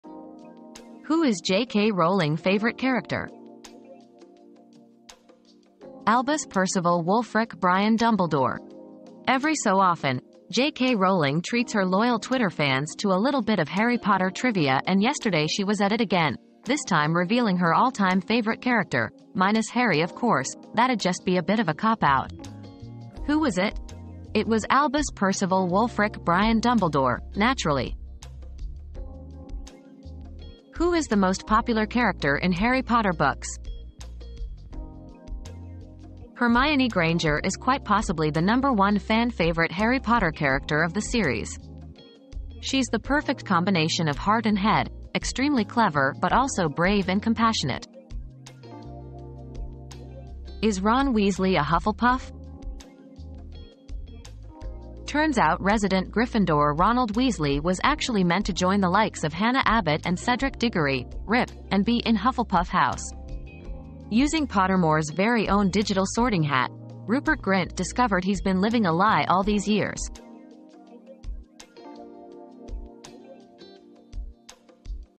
Who is J.K. Rowling favorite character?
Buzzfeed Which Character Are You Harry Potter

00:00 - Who is J.K. Rowling favorite character?
00:30 - Who is the most popular character in Harry Potter books?
00:50 - Is Ron Weasley a Hufflepuff?